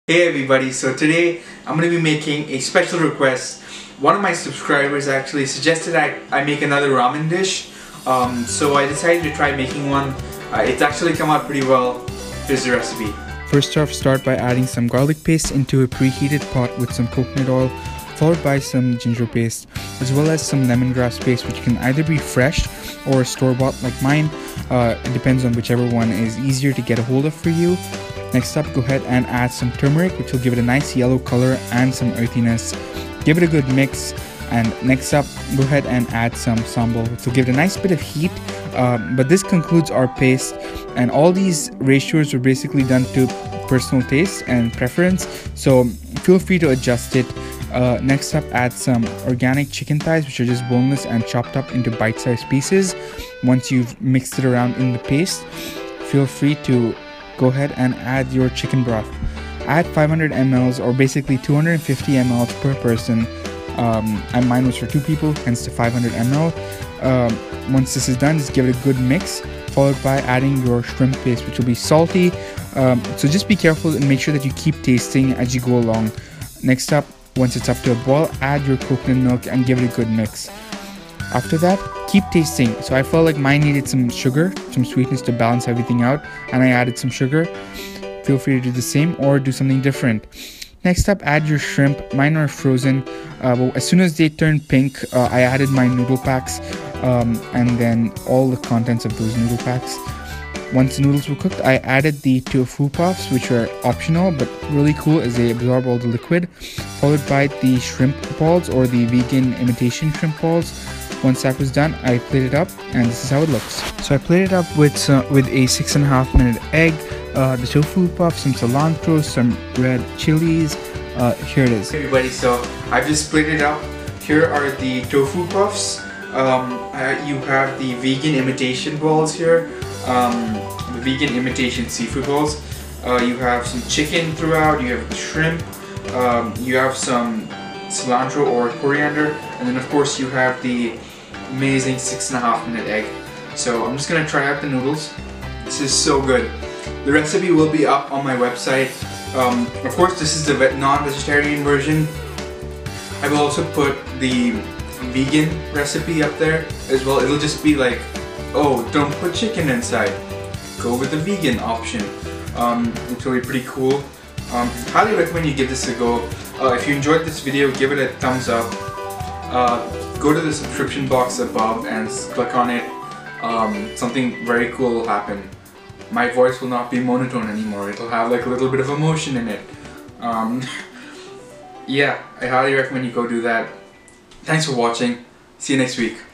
Thai Ramen | Calvin Serrao
As per a special request from a subscriber, here is a Thai Ramen dish which is spicy, salty, sweet, sour and has the perfect amount of umami.

Recipe available here at my website!

Logo Credits to Nivea Serrao (@NiveaSerrao)

Feel free to add me on social media!
Facebook Calvin Serrao